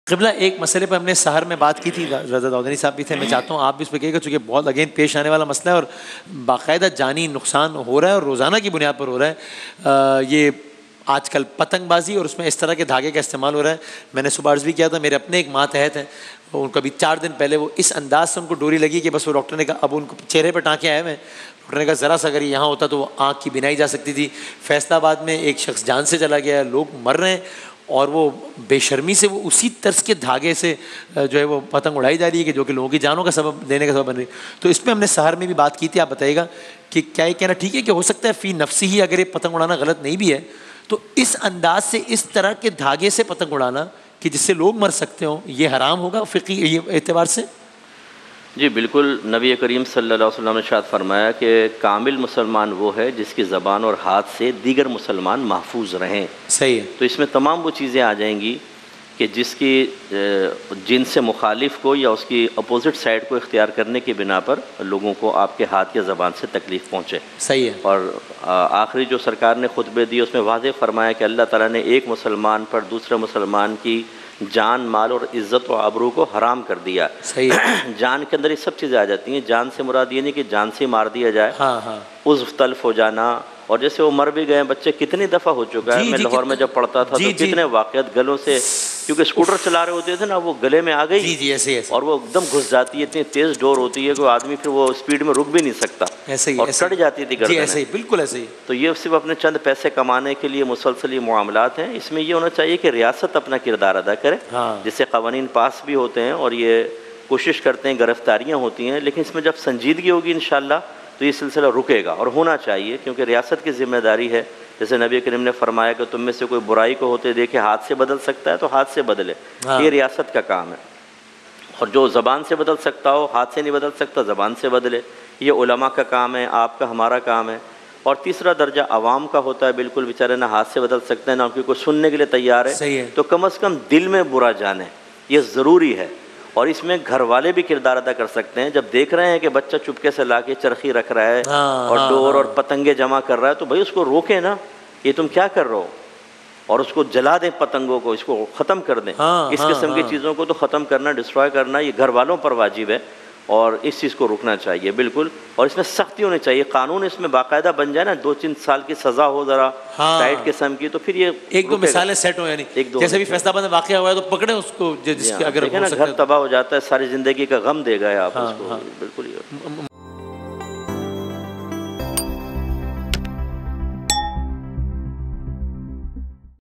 Patang Baazi aur Mulk ka Kanoon | Mufti Muhammad Akmal
Guest:
Mufti Muhammad Akmal,
Allama Muhammad Raza Dawoodani.

An informative segment with a Q&A session that features religious scholars from different sects who will share their knowledge with the audience.